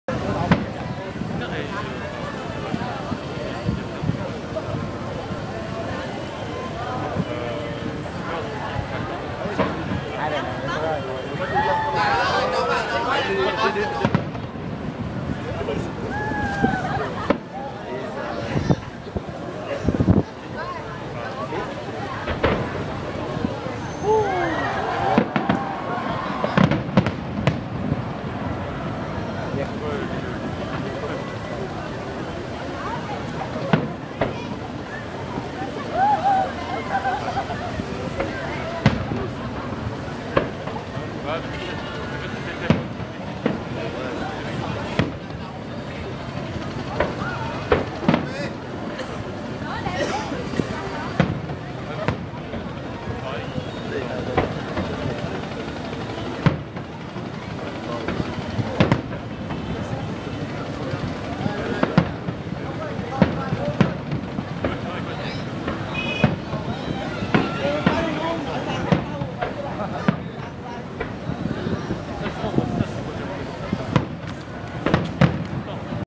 Tet 2013 in Hue, Vietnam (4/7)
Vuurwerk afgestoken tijdens het Vietnamees nieuwjaar 2013.